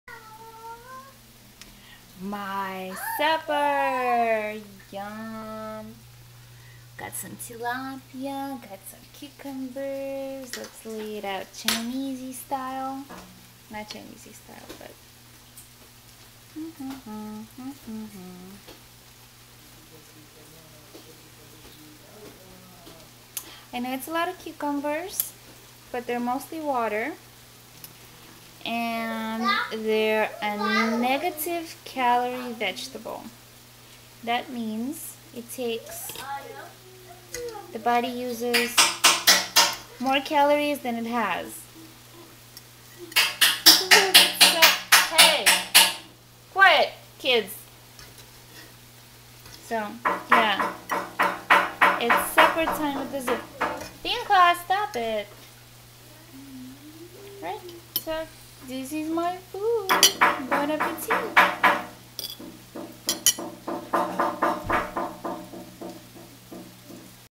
hcg - Tilapia & Cucumbers meal
I couldnt finish all those cucumbers, guys! I was just super hungry while chopping those up.

Emeril's Bayou Blast spice mix is the bomb for tilapia!

Dont mind the noise in the back, kids were just being themselves. :)